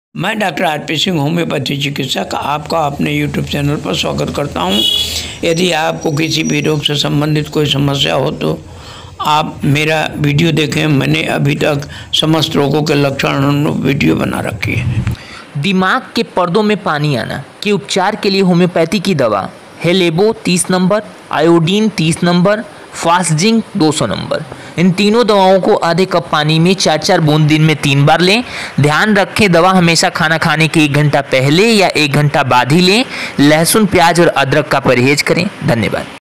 दिमाग के पर्दो में पानी आना (Hydrocephalus) का होम्योपैथिक उपचार डॉआरपीसिंहDrRPSingh
दिमाग के पर्दो में पानी आना (Hydrocephalus)
का होम्योपैथिक उपचार
डॉआरपीसिंहDrRPSingh
Homeopathic Medicine
हेलेबो, आयोड, फॉ जिंक ।